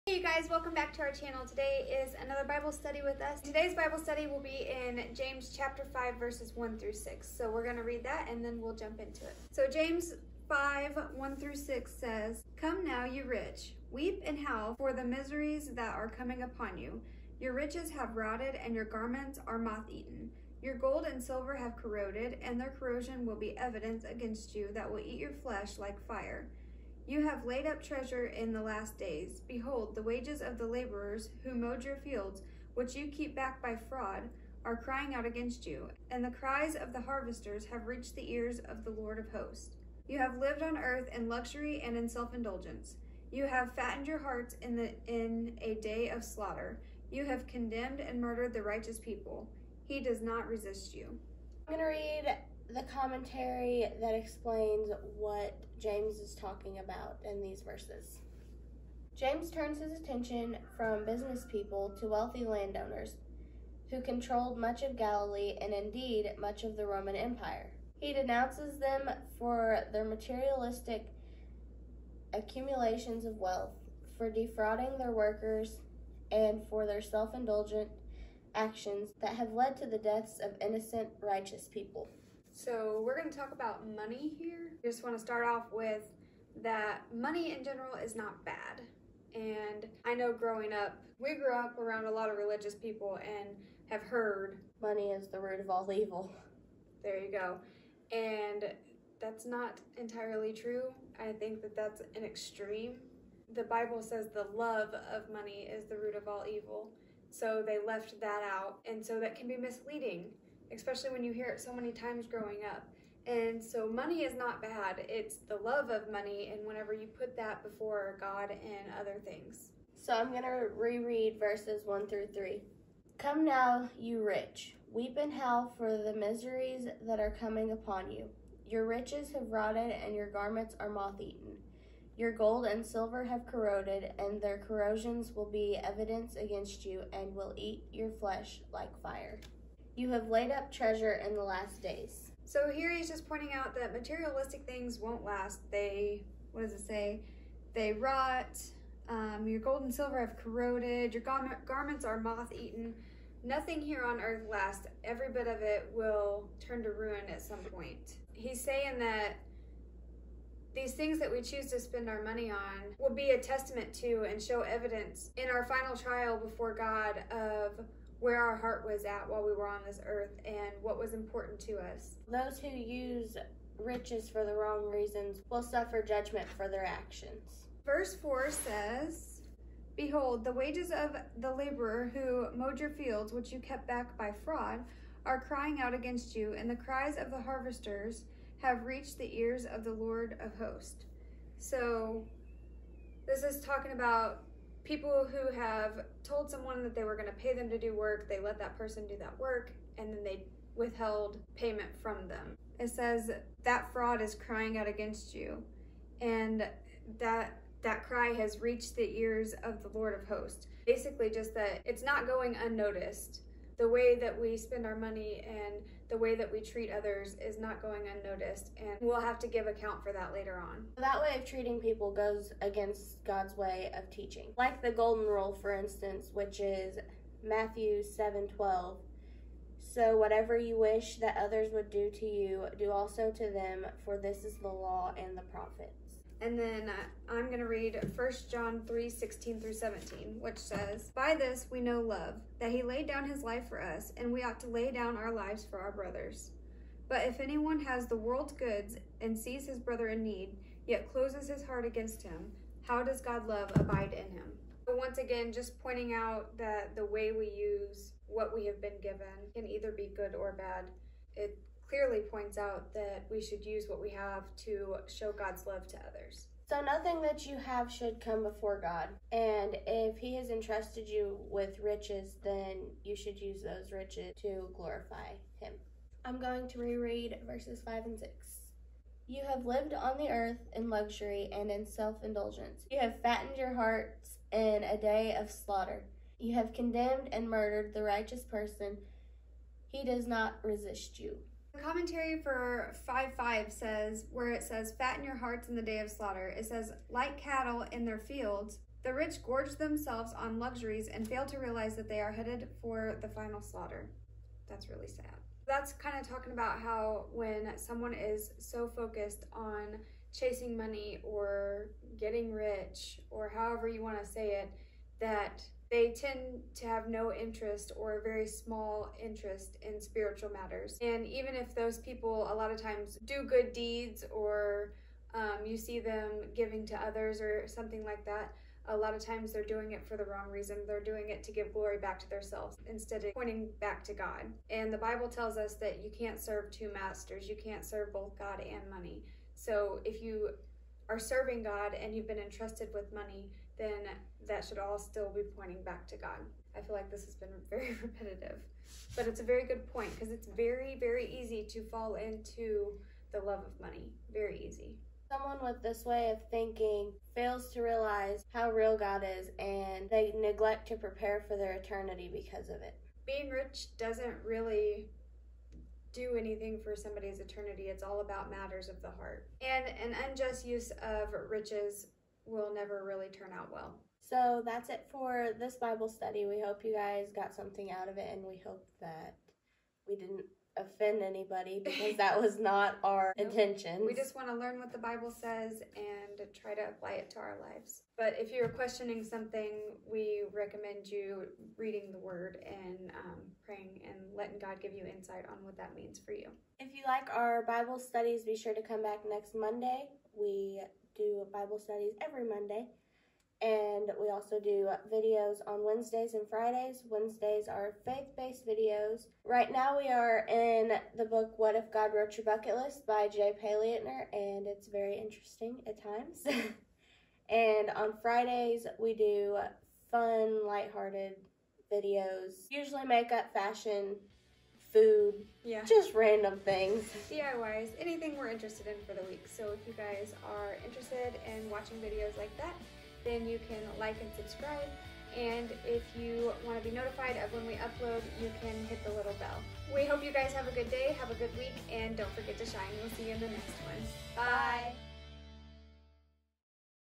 Bible Study With Us! 1 John 5:1-6 ~ Women’s Bible Study | adustydiamond
This is a Bible study from James 5:1-6. In these verses James is explaining that chasing money can cause us to be worried about the wrong things! We can become consumed about what all we can gain while here on Earth. We get so caught up in what we want and what we think we need that we forget to use what we have to show God’s love to others. Not to mention how little we consider our eternity while we are “living it up” in the here and now. We should use what God has given us to point back to him. For our eternity and for the eternity for others!